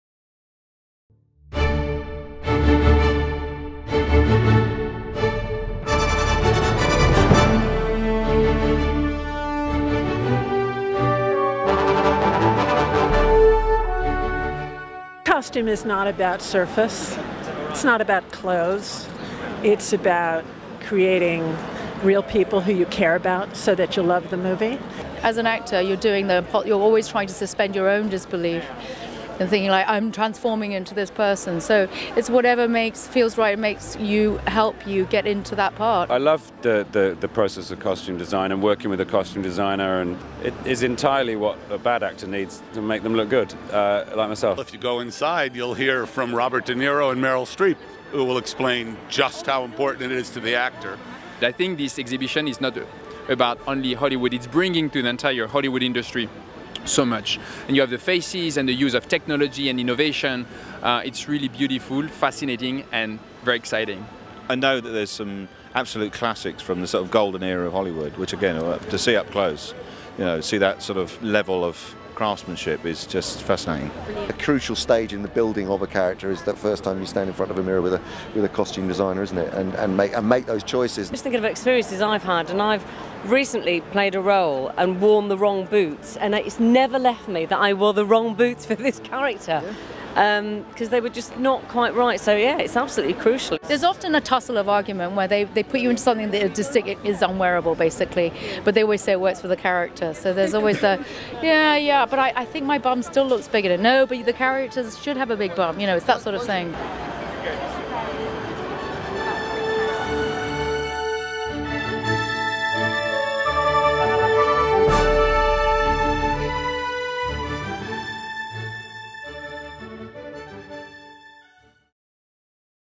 The Establishing Shot: THE V&A's HOLLYWOOD COSTUME PREMIERE NIGHT
The Establishing Shot: VICTORIA AND ALBERT MUSEUM HOLLYWOOD COSTUME EXHIBITION PREMIERE NIGHT INTERVIEWS with Deborah Landis, Helena Bonham Carter, John Landis, Simon Pegg, Harry Winston, Fashion, Style, Bill Bailey,  Andy Serkis, Lorraine Ashbourne and Michael Kaplan